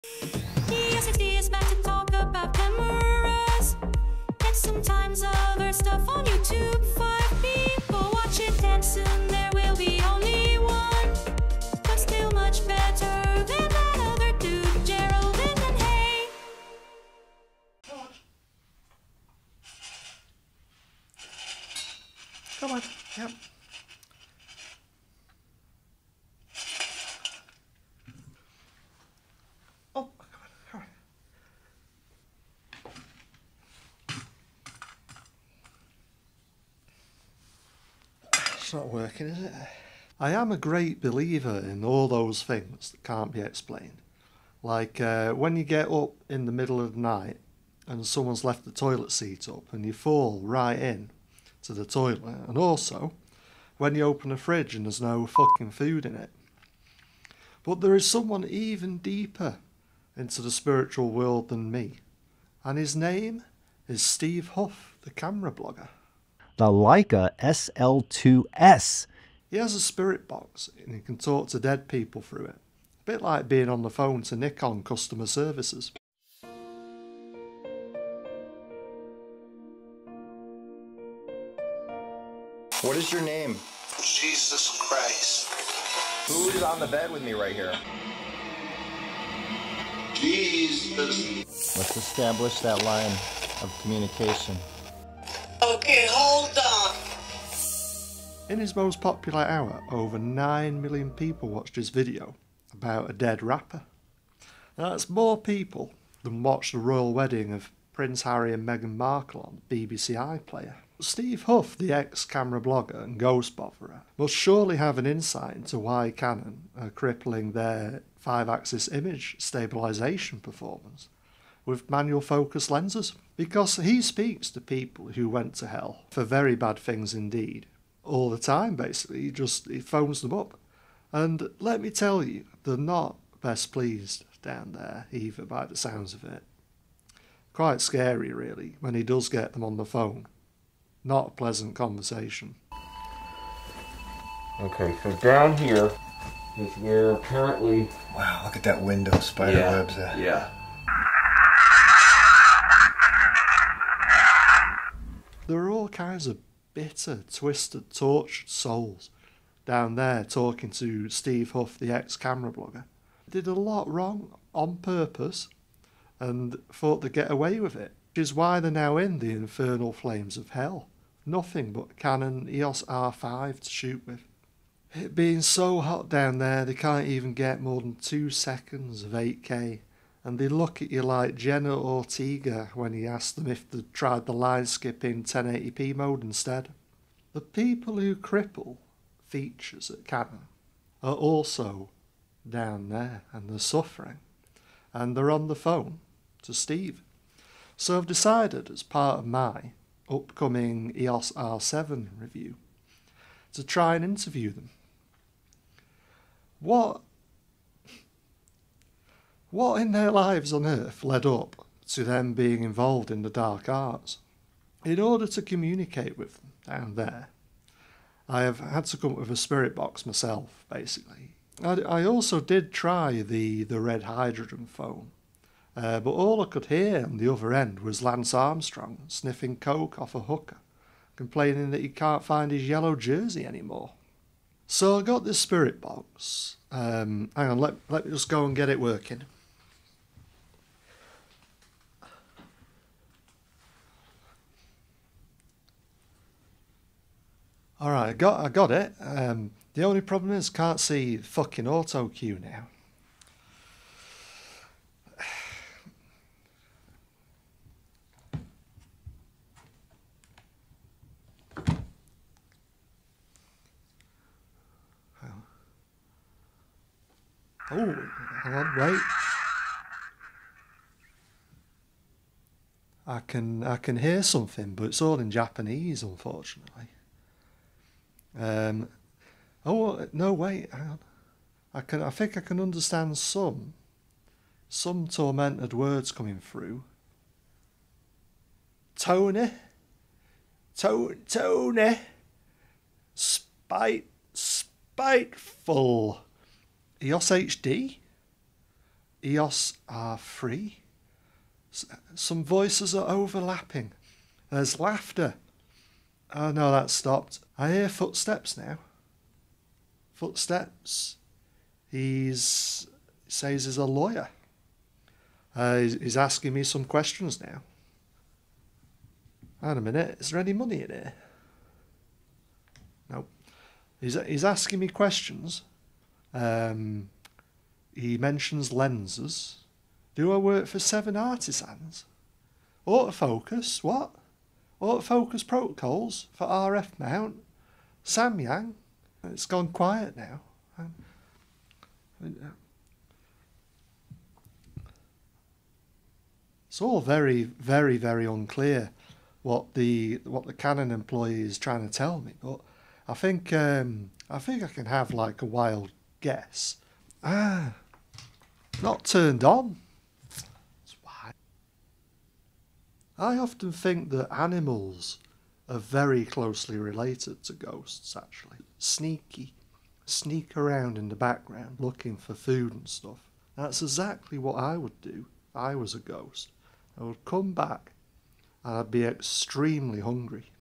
Paranormal Activity at Canon?
Mysteriously 5 axis image stabilisation performance is disappearing with non-Canon lenses on the latest EOS R3 and EOS R7. What's going on? EOSHD reaches beyond this life into the spirt world to find some answers.